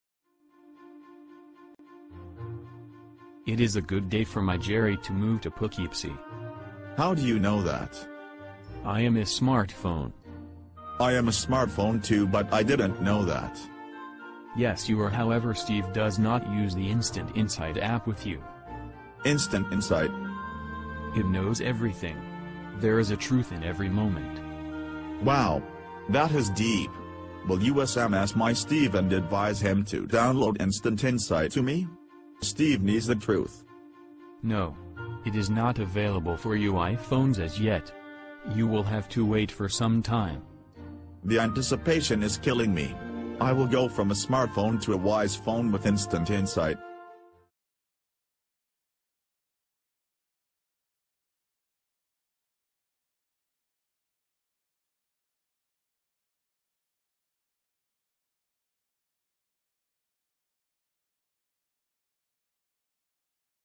Instant Insight on Android and iPhone
Instant Insight (Prasna Astrology) is an amazing mixture of ancient and modern Vedic predicting techniques. This form of astrology is also known as 'Prasna Shastra' (Sanskrit, Prasna means question), since it involves a person asking a specific question about any aspect of his or her life.

The Ancient Technology of obtaining solid, reliable guidance from the great beyond is now accessible to you using Modern Technology in the form of Instant Insight.

Instant Insight is now available on Android phones for download for FREE.